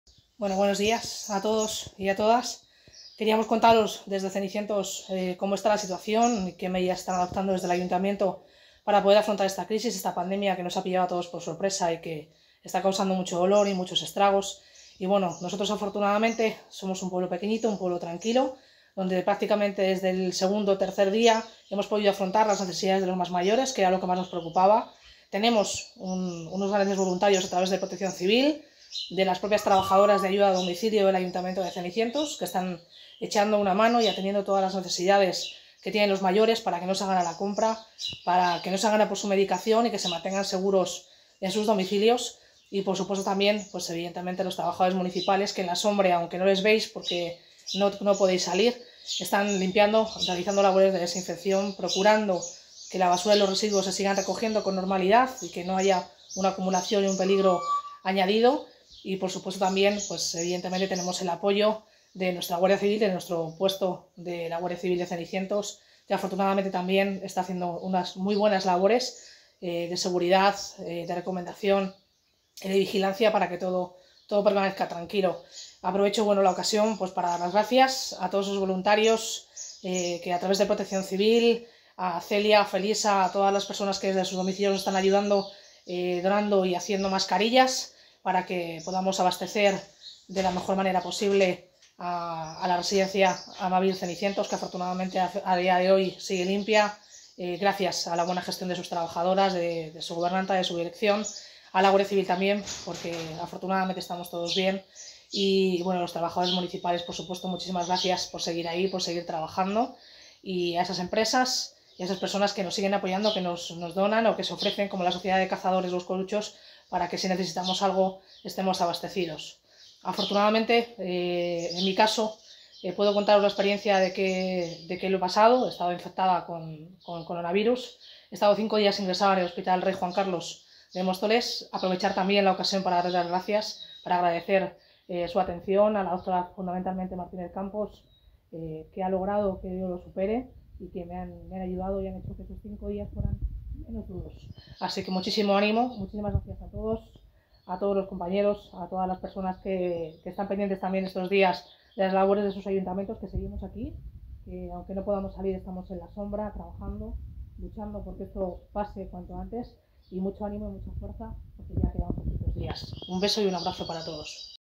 COVID19. Nuestros Ayuntamientos. Natalia Núñez, alcaldesa de Cenicientos.
Actuaciones para paliar las consecuencias del COVID19. Abril 2020